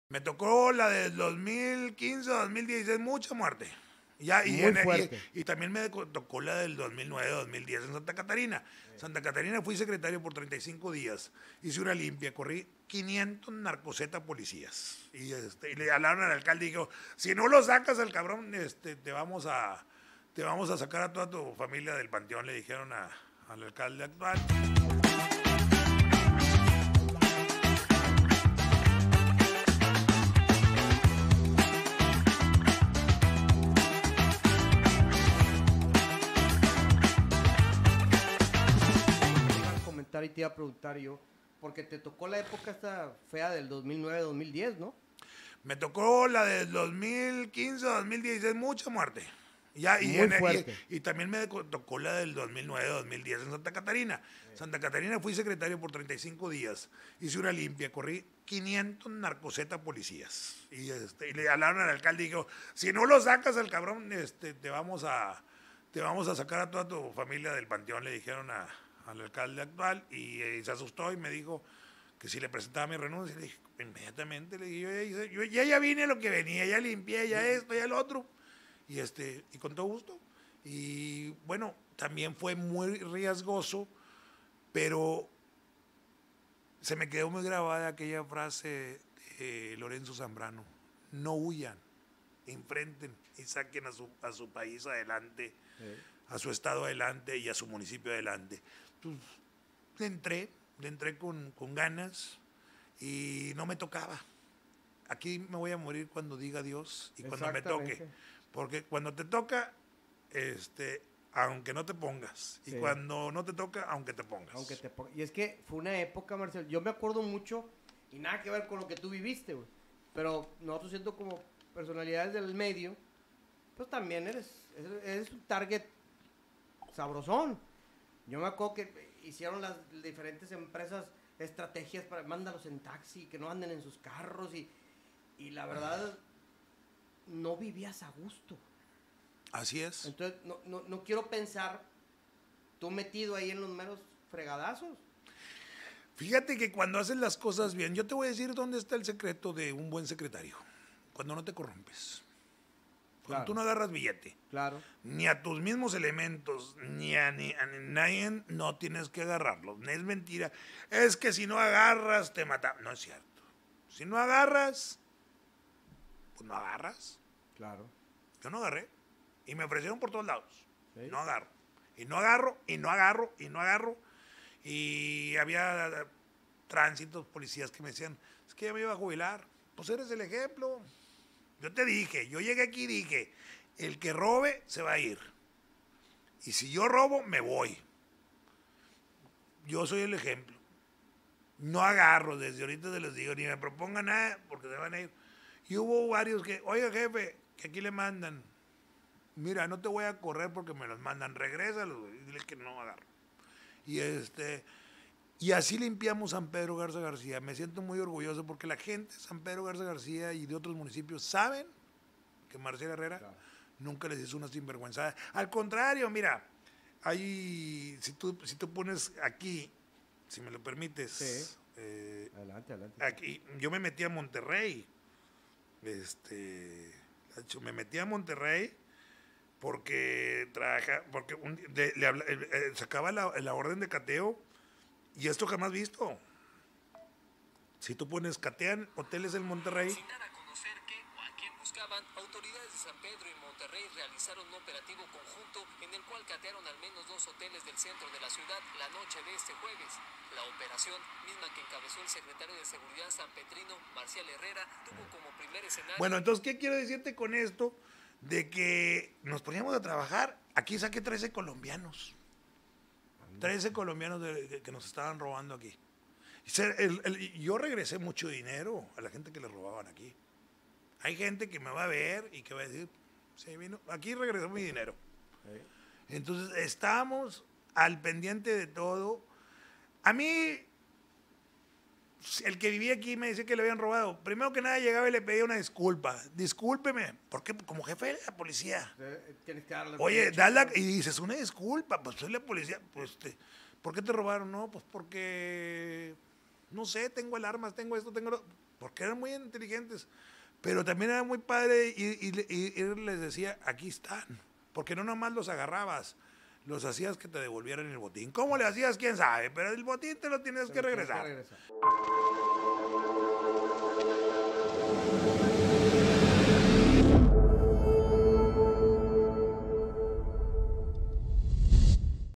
Corrí a 500 narcopolicías / Un Podcast + con Lacho Gutiérrez invitado Marcial Herrera
Un Podcast + con Lacho Gutiérrez invitado Marcial Herrera
SiverTv Digital Studio